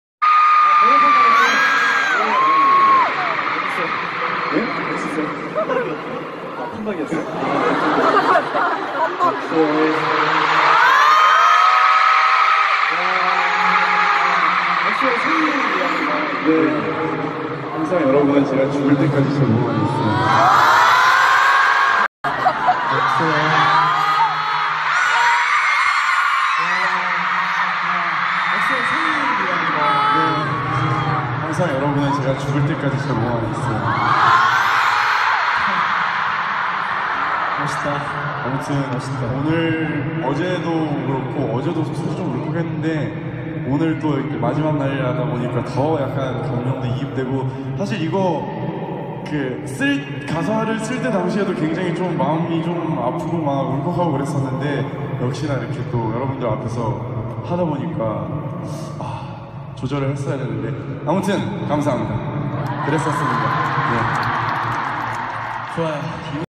(171126) Chanyeol's New Tattoo Is Real - The ElyXion In Seoul
Chanyeol : You are in my body until I die

chanyeol’s new tattoo is real, it’s a proof how special exo-ls are to him like we really became part of his life, to the point that he tattooed aeri’s birthday on his arm, and it will forever remind him of us